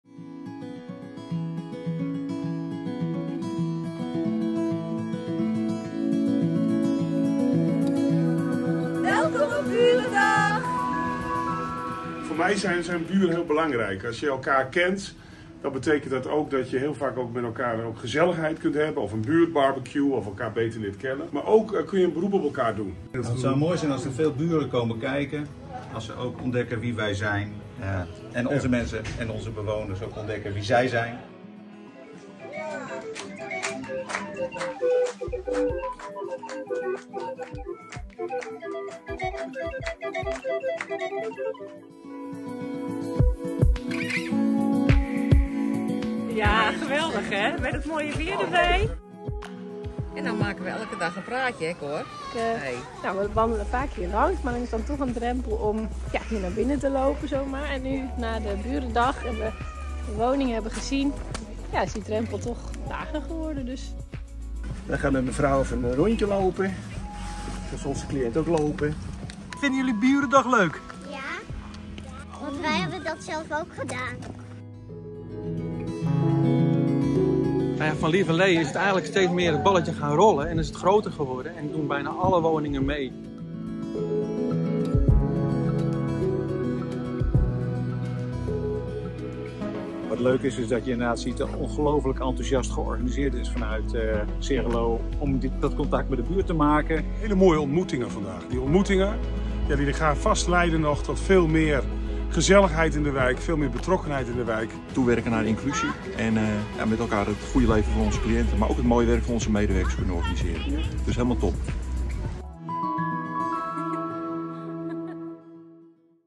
's Heeren Loo Burendag | 2023
We vierden Burendag bij onze woningen in Julianadorp. Buren kwamen op bezoek bij 's Heeren Loo en dat vierden wij met gezellige activiteiten, ontmoetingen en een kop koffie.